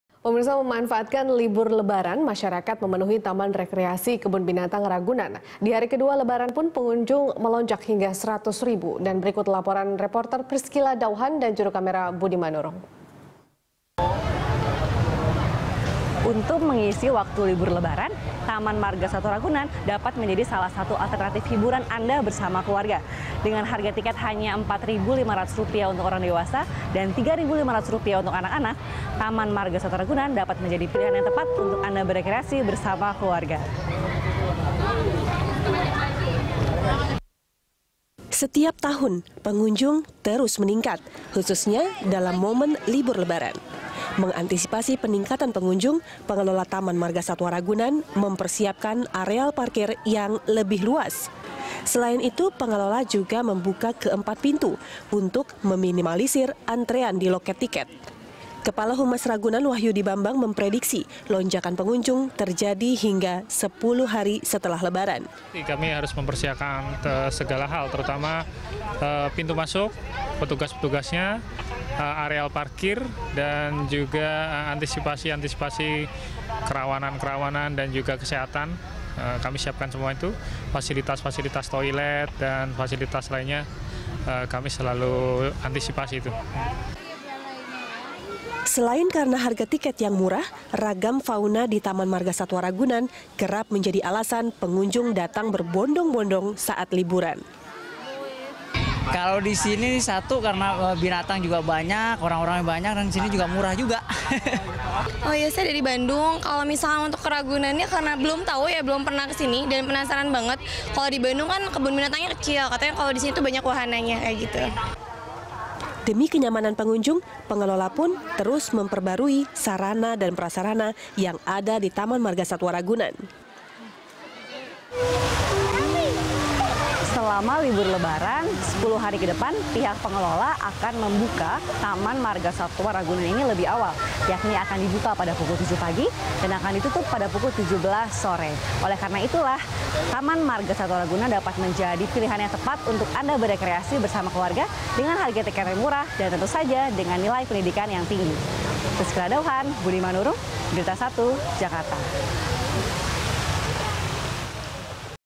Pengunjung Membludak, Pengelola Ragunan Siapkan Parkir Luas
Memanfaatkan libur Lebaran, masyarakat memenuhi taman rekreasi kebun binatang Ragunan, Jakarta Selatan. Di hari kedua Lebaran, pengunjung melonjak hingga 100 ribu orang. Alhasil pengelola Taman Margasatwa Ragunan harus menyiapkan lahan parkir yang lebih luas untuk menampung kendaraan para pengunjung yang membludak.

Facebook.com/BeritaSatuTV
Youtube.com/BeritaSatu